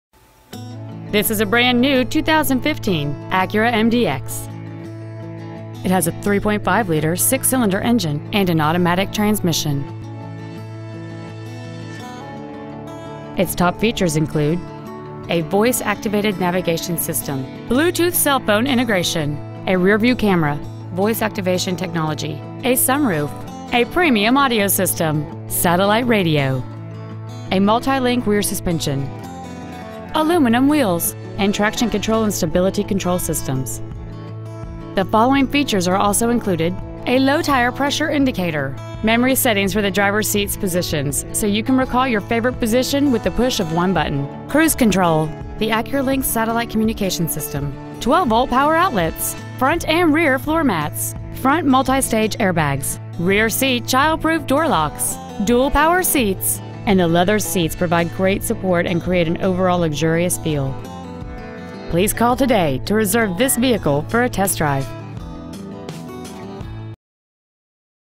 2015 Acura MDX Laurel MD 20724
We have been honored to serve the Laurel MD area , we promise that your experience at our dealership will exceed your expectations ! Year : 2015 Make : Acura Model : MDX Engine : Premium Unleaded V-6 3.5 L/212 Trans . : Automatic Exterior : White Diamond Pearl Interior : Parchment Trimmed Tischer Acura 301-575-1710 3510 Fort Meade Road Laurel , MD 20724